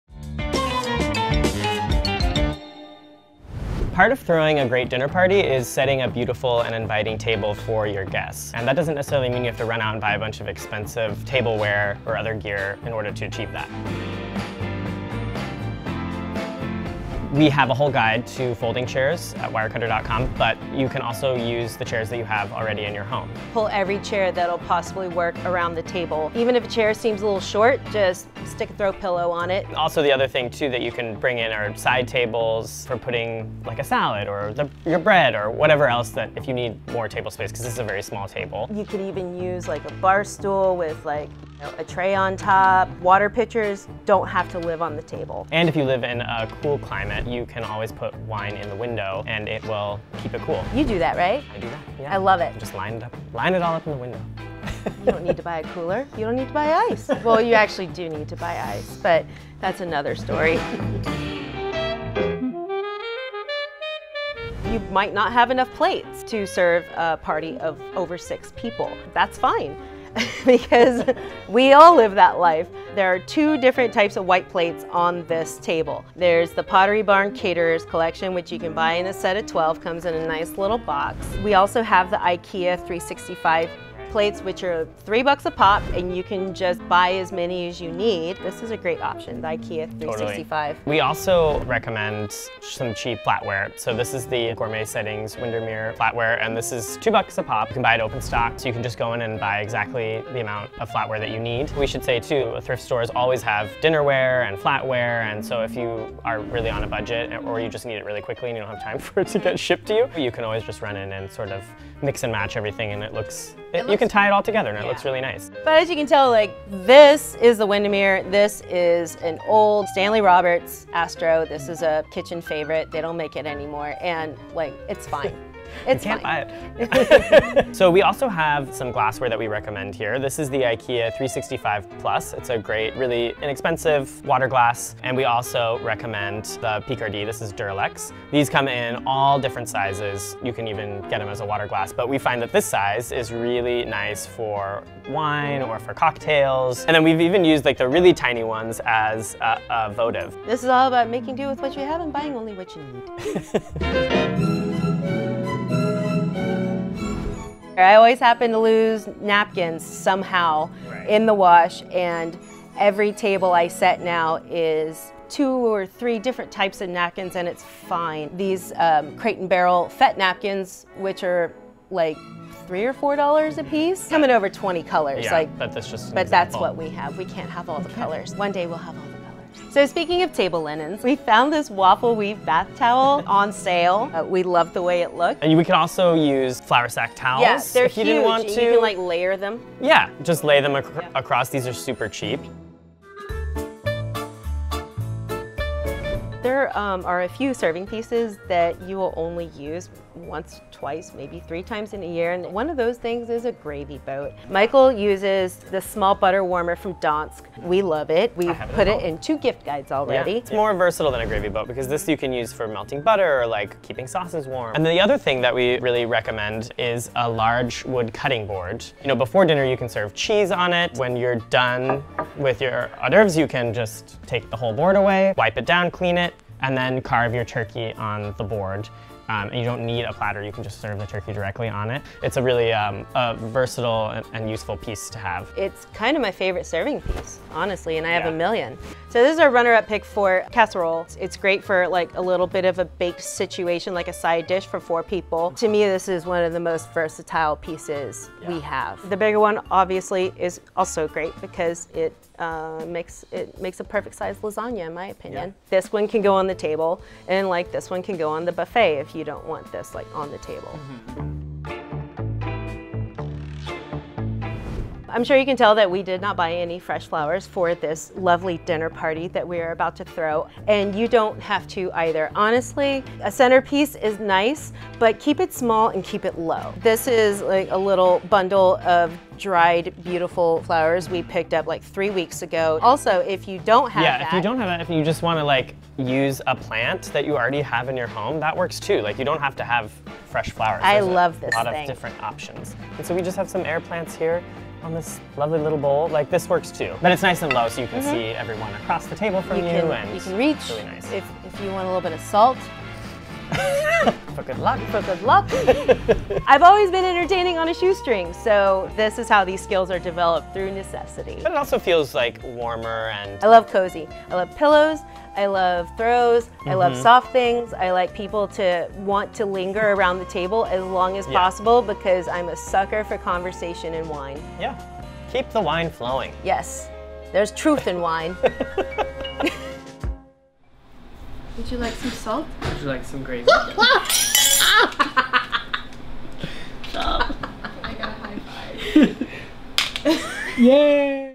Tips for Setting a Beautiful Table On a Budget
A thoughtful table setting can contribute to a warm and inviting Thanksgiving or dinner party just as much as the food. Pulling it off doesn't require fancy matching plates or elaborate decor. In this video, Michael and Lesley share plenty of great ways to make the most of what you have, with some affordable Wirecutter picks if you're in need of supplies.